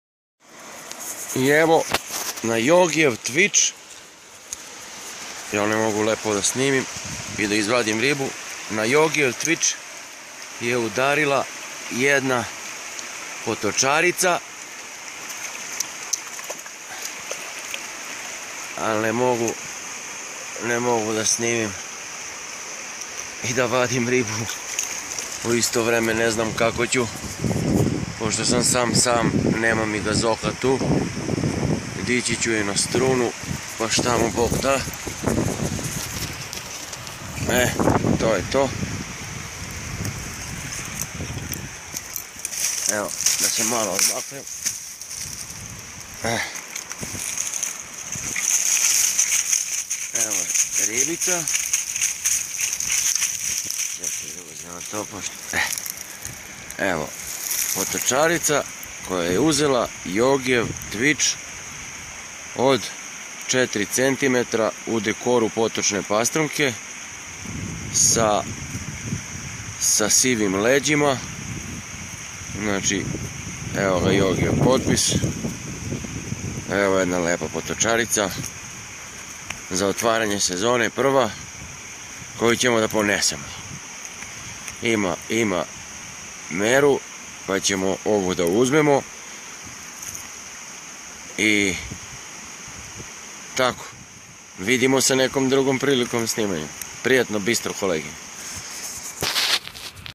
Novi Yogi twich u akciji - spinning trout - forellenangeln - fishing - pêche - рыбалка - pesca fiske
lepa potocarica na neponovljivi i najlovniji twich vobler. naravno napravljen u radionici gde nastaju svi YOGI vobleri...:)...bistro...

fishing - angeln - pêche - рыбалка - pesca – fiske

spinning trout - forellenangeln